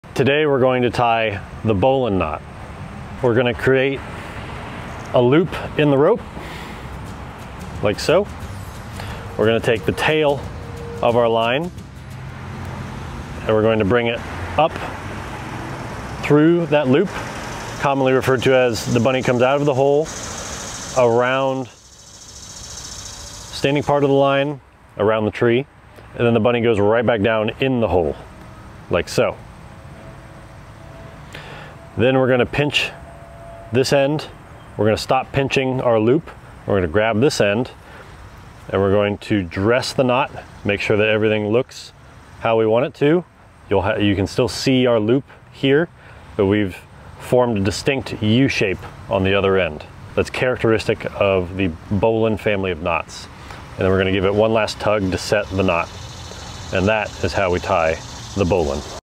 How to tie the bowline knot
How to tie a bowline knot by ISA Certified Arborist Jacob Nau, our training and development manager! Check out our YouTube channel for a more in-depth, informational video on the bowline knot!